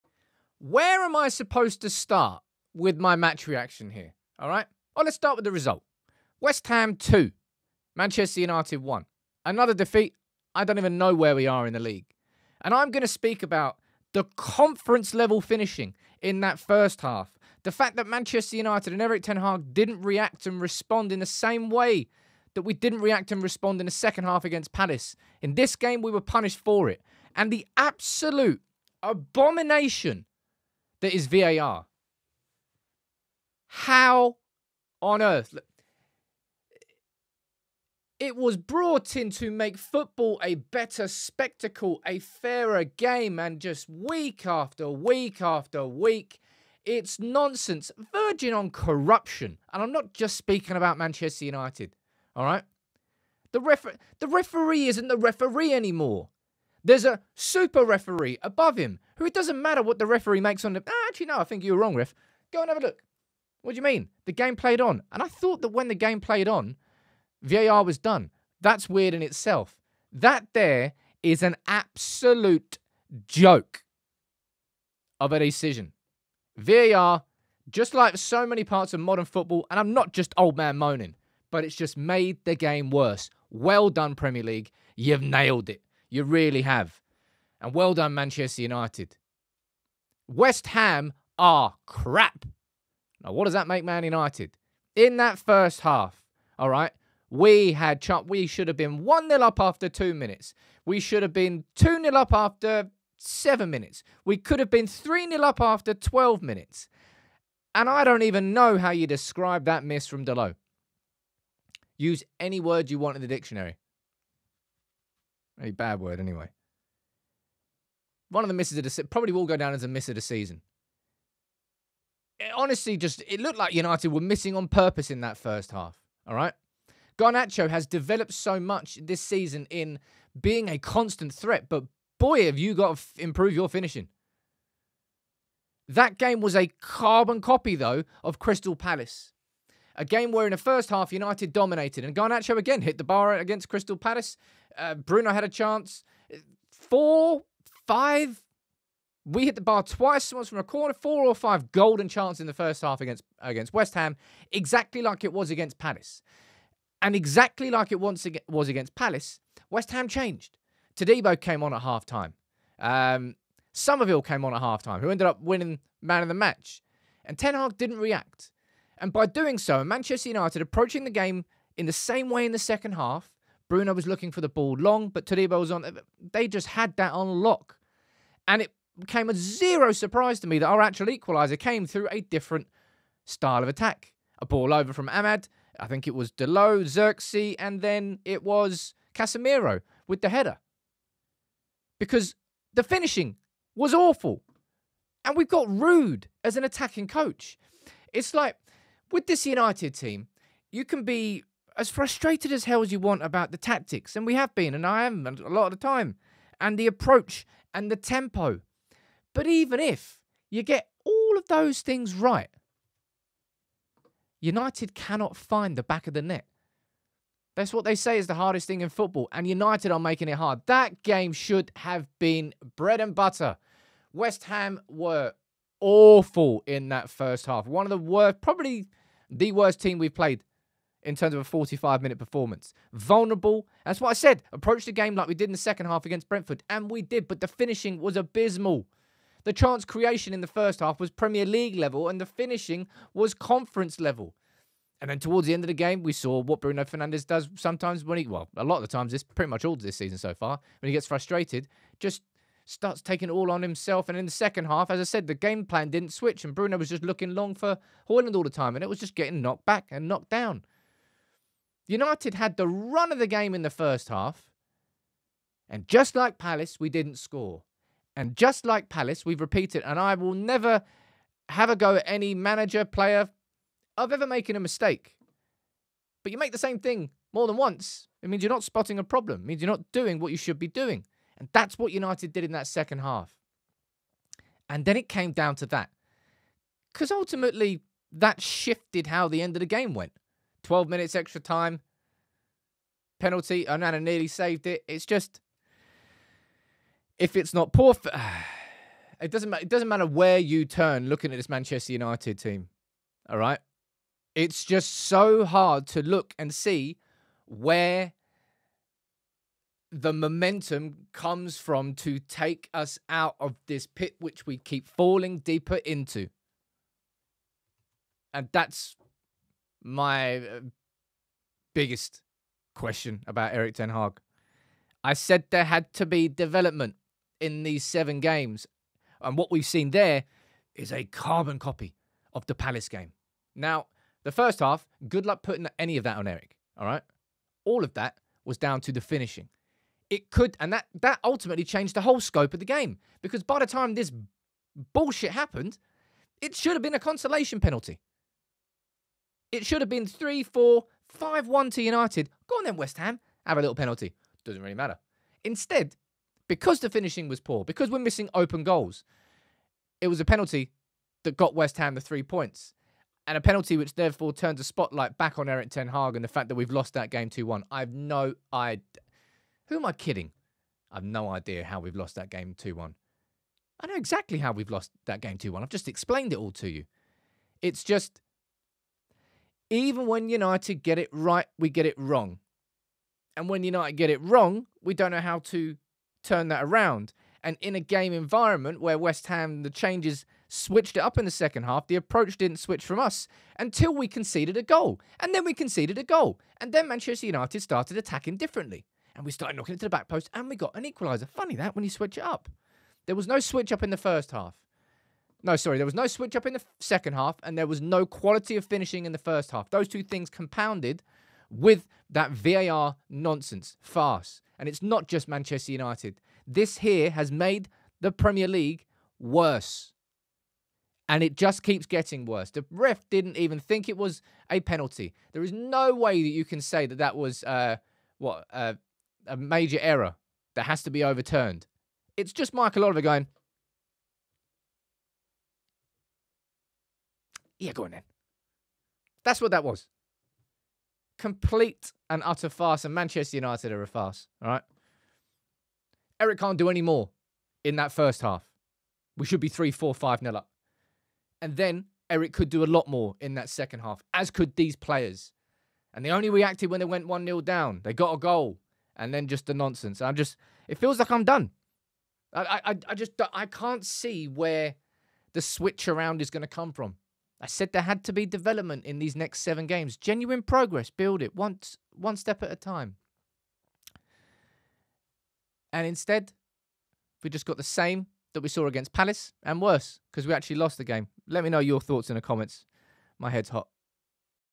I Am Done & Exhausted...VAR Is A Complete Farce | WEST HAM 2-1 MAN UTD
Conference level finishing, no adaption in the second half from Manchester United and a Michael Oliver VAR farce? Yeah, I am done. It's exhausting.